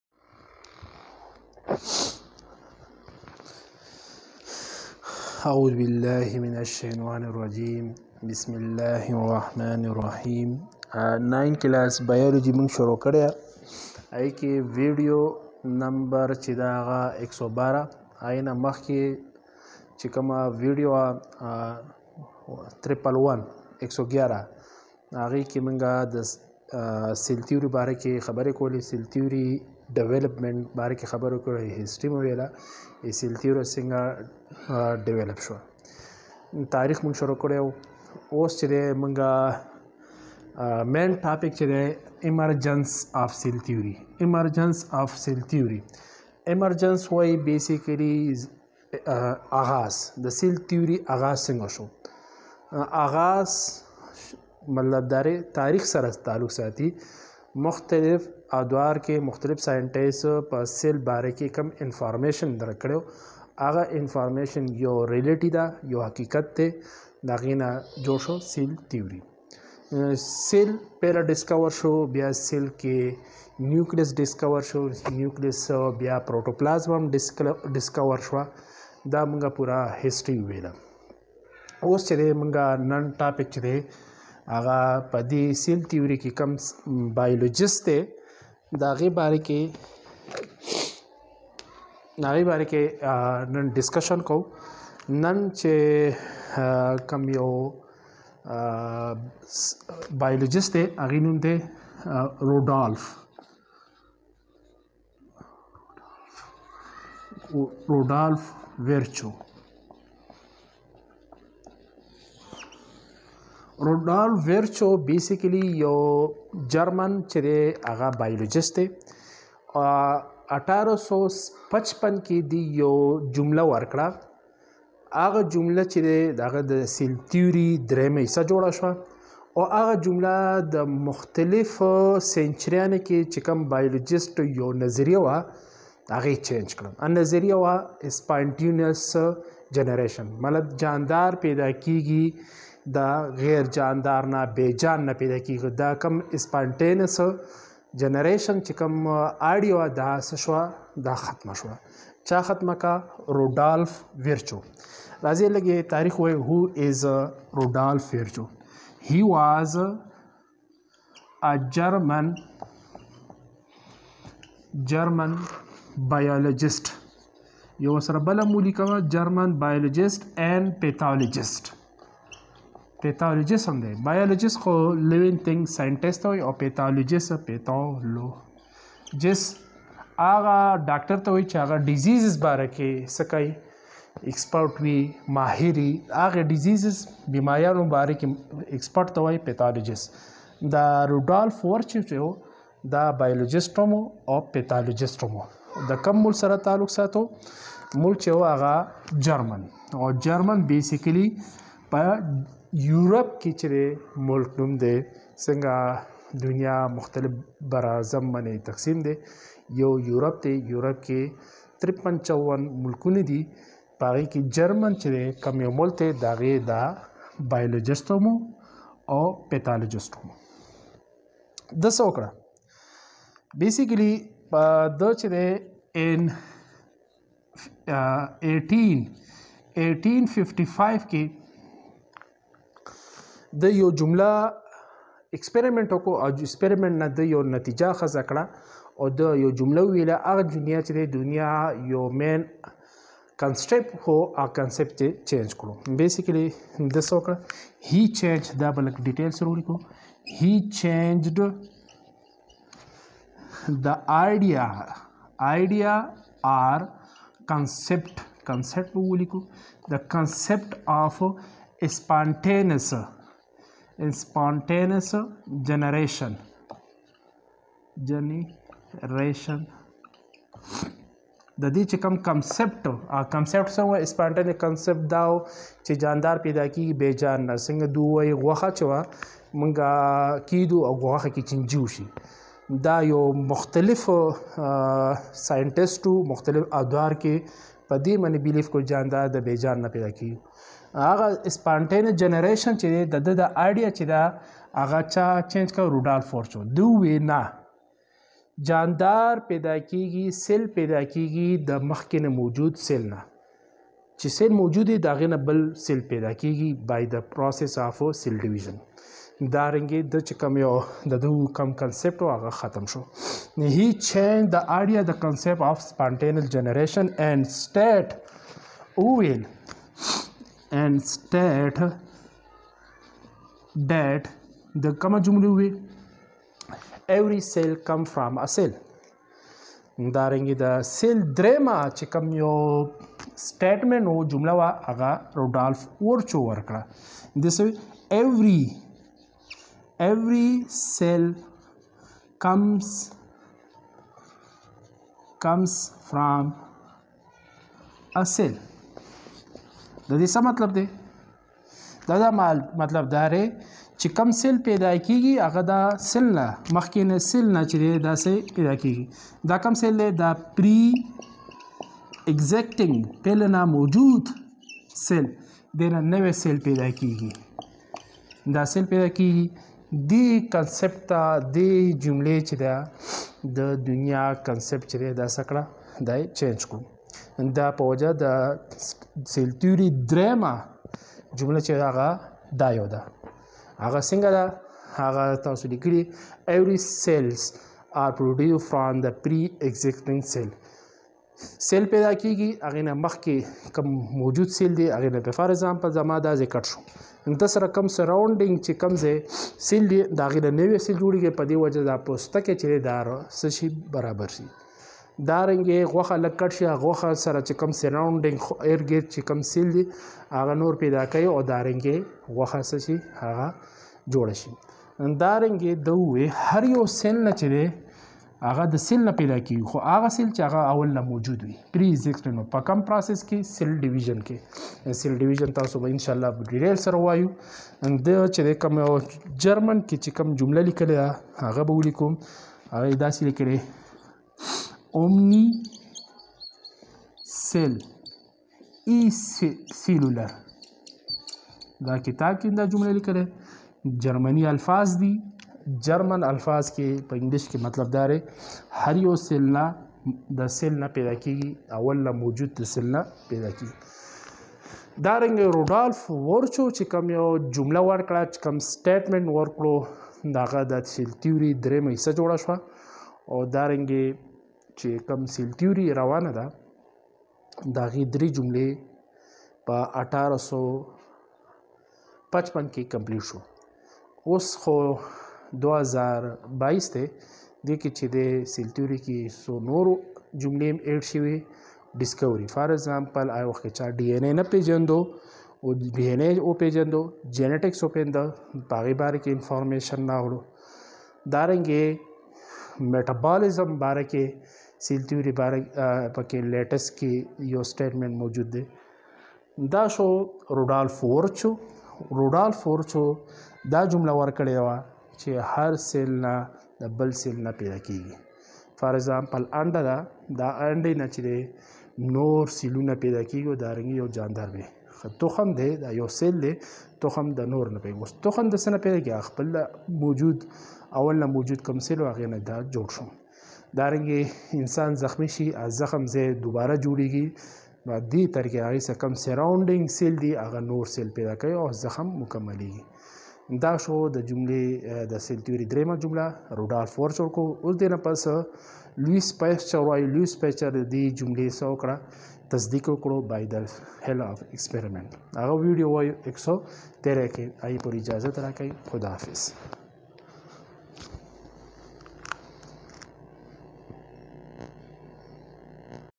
Emergence of cell theory | Rodulf Virchow | 9th class biology | in Pashto
Emergence of cell theory
Rodulf Virchow
He was a German biologist and pathologist. He changed the idea of a spantaneous generation and said that every cell comes from a pre existing cell.